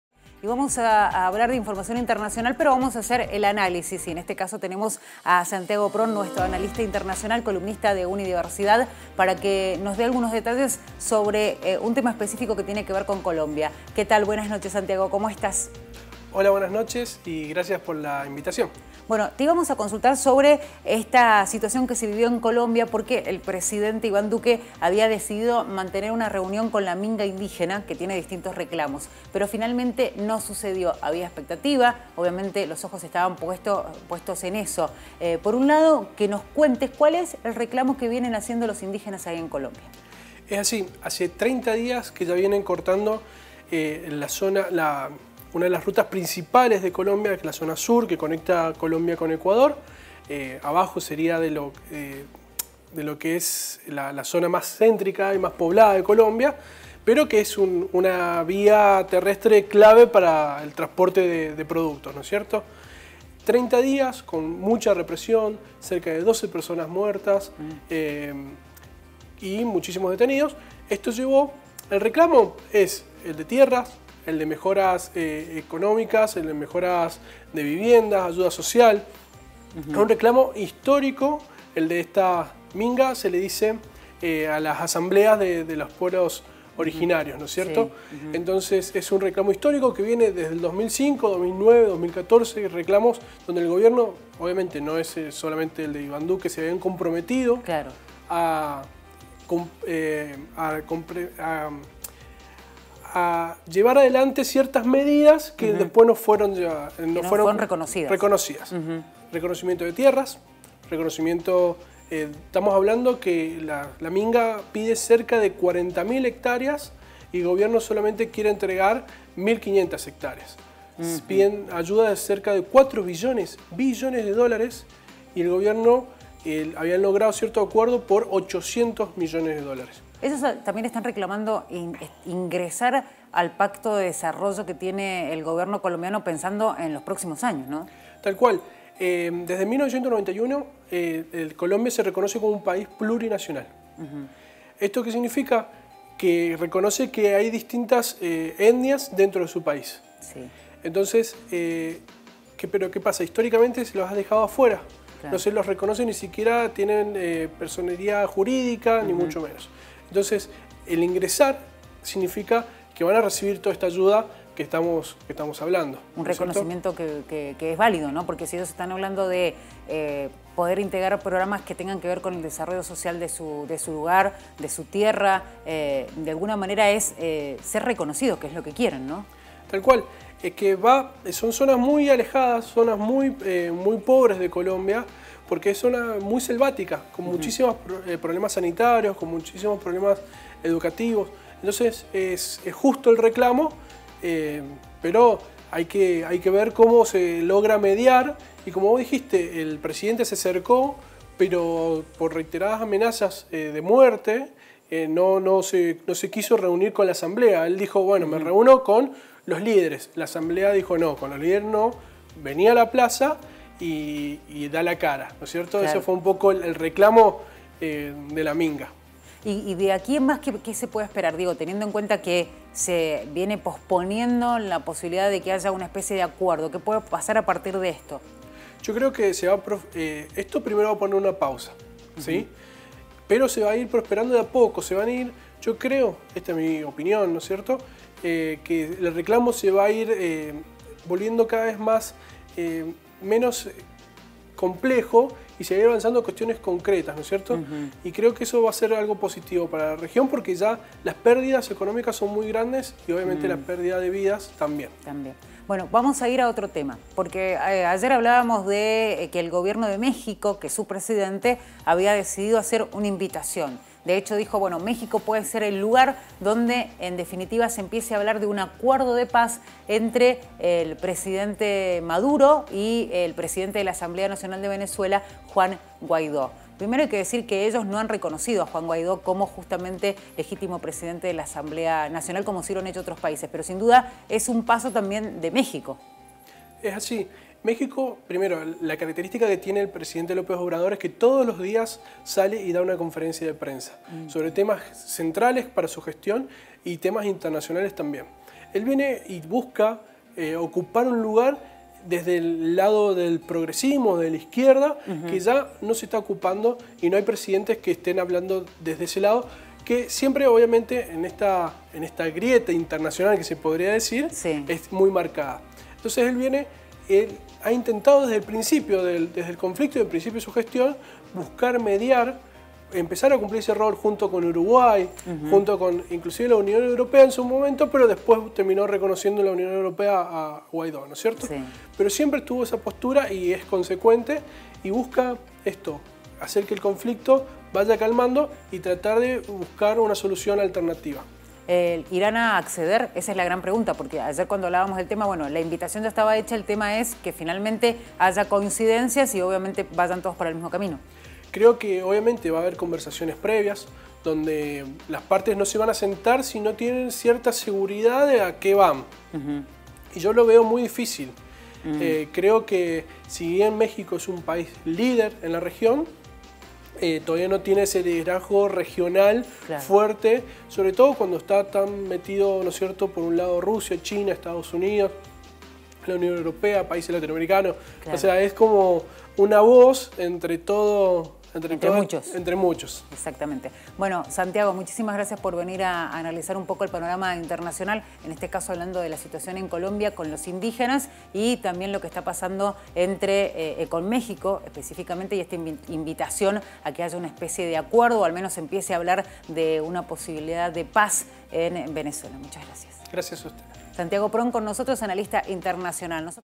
Reclamo histórico de la minga indígena en Colombia
Santiago Pron, columnista de temas internacionales, habló de la tensión en el país sudamericano y de la actitud del presidente Duque. También contó que Lopez Obrador mediará el conflicto en Venezuela.